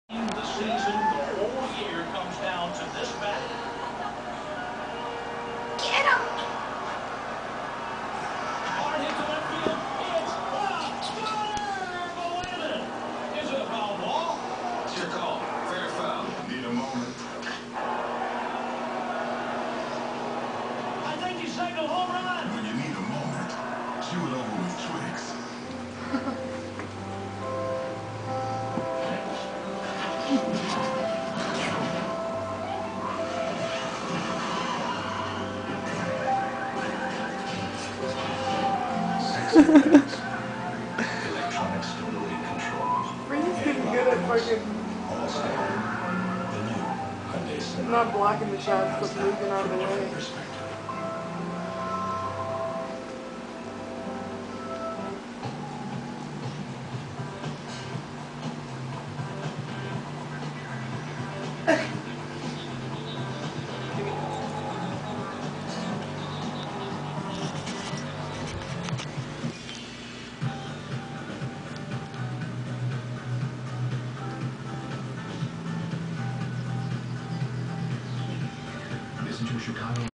Ferret vs Cat - Part 5
This is the long awaited Part 5 of the epic battle continuing between Frankie the Ferret and Phil the Cat!  Phil is a Bombay breed kitten and Frankie is a Sable ferret.  Please excuse the mess... I was in the process of moving into a new place!